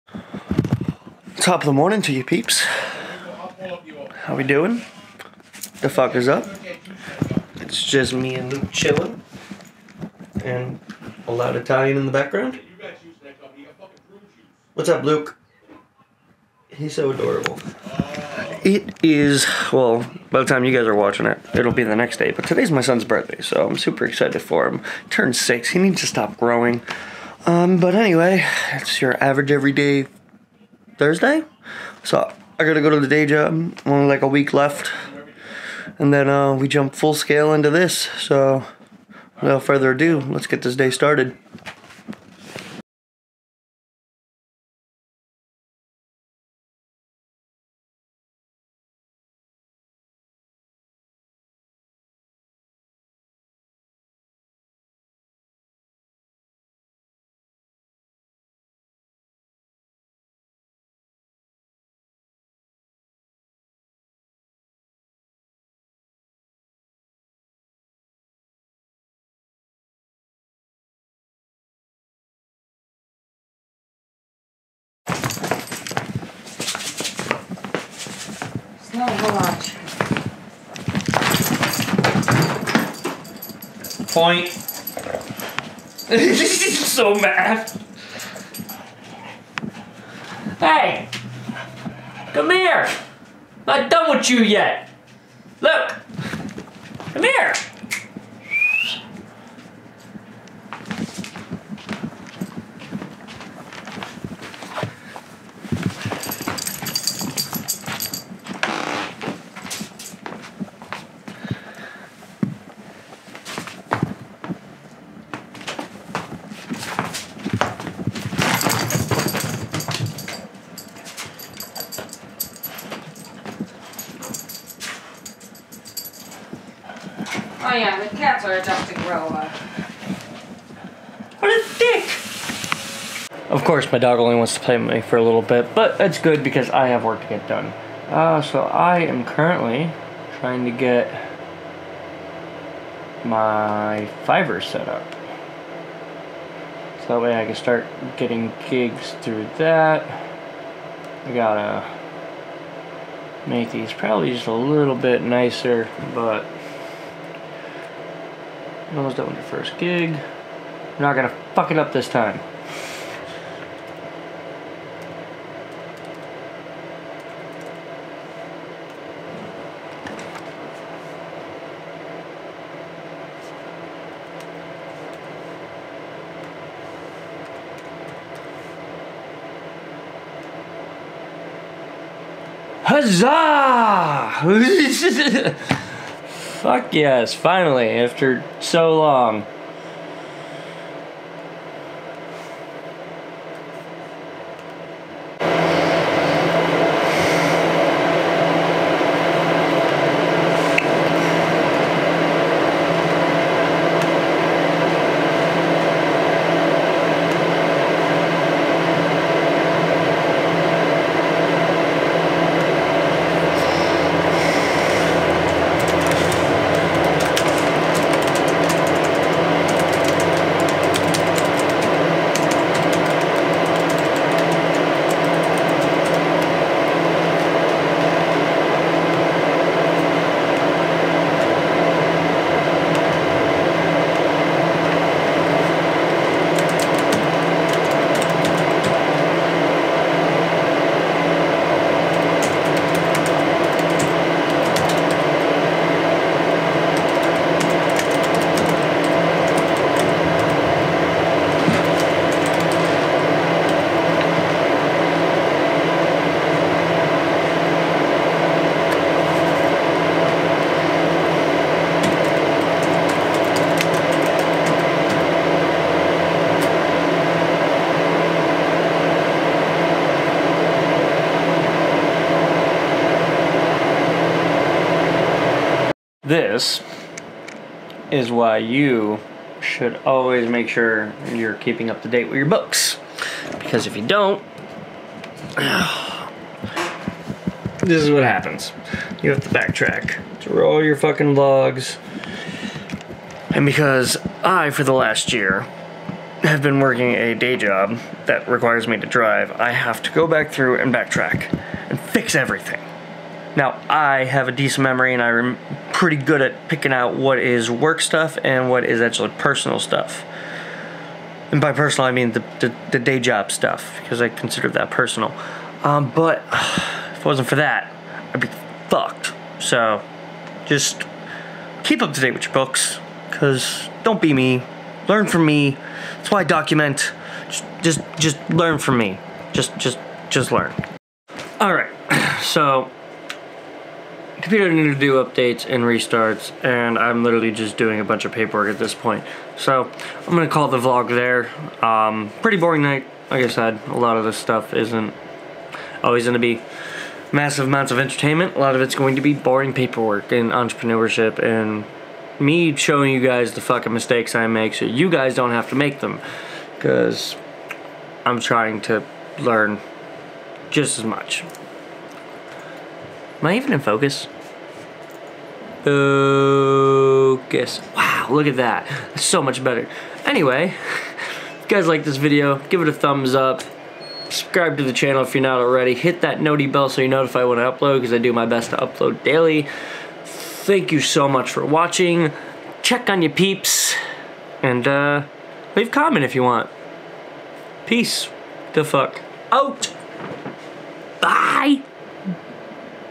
Learn from my mistakes
Yoooo! What Dafuq is up!? The more I play catch up on my books, the more I realize how bad I have been with taking this seriously. So, watch and learn from my mistakes and if you're making the same ones, what are you doing!? With that being said, please check on your peeps and I'll see you guys and gals in the next video!

Business: P.F.D. Media